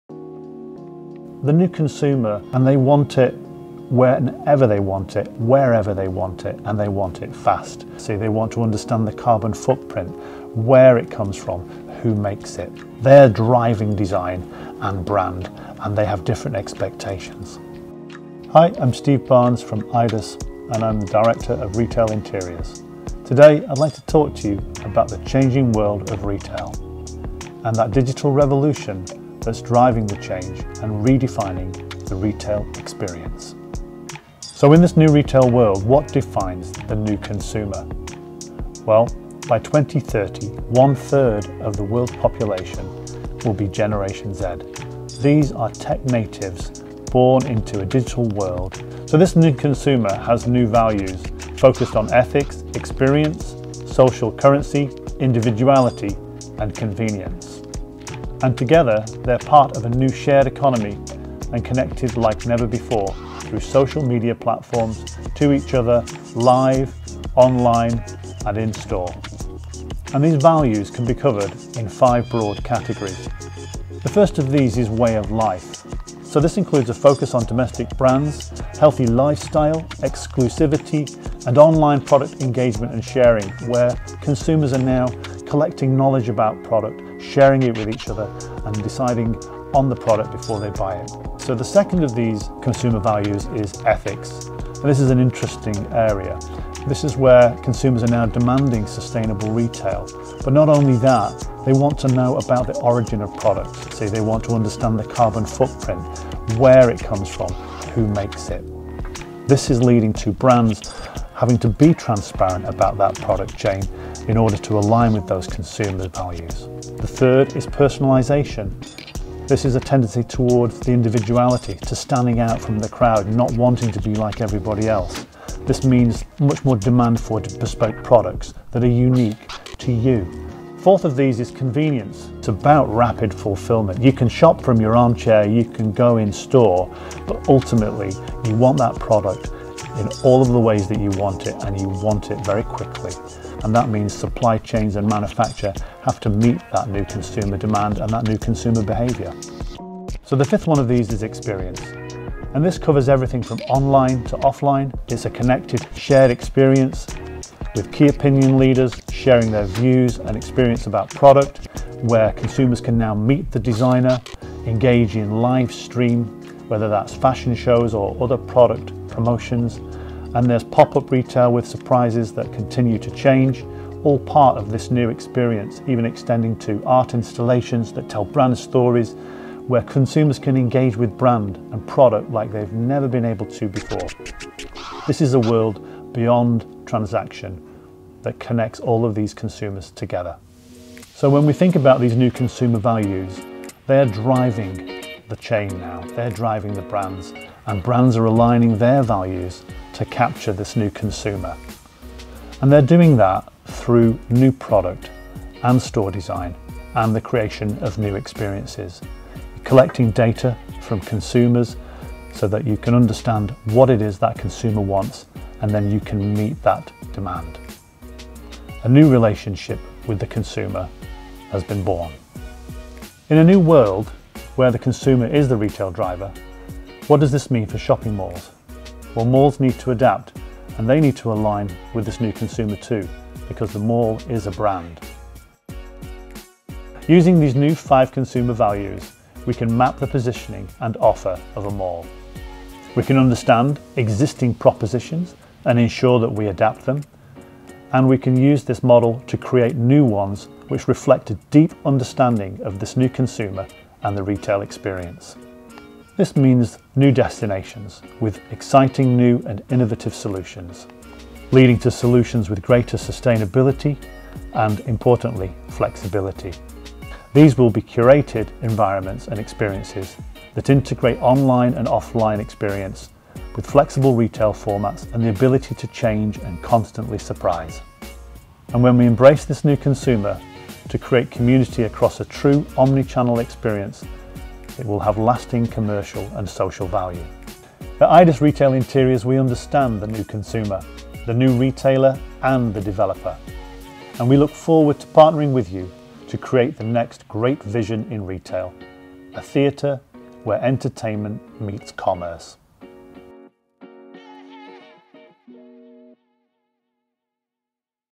Explore the Future of Retail Design with Steve Barnes
What defines the new consumer and what are the new consumer values? In a new world where the consumer is the retail driver, what does this mean for shopping malls?

With over 30 years’ experience, Steve Barnes, Executive Director leading Aedas Retail Interiors, will talk about how digital revolution is driving the change and redefining the retail experience through this video.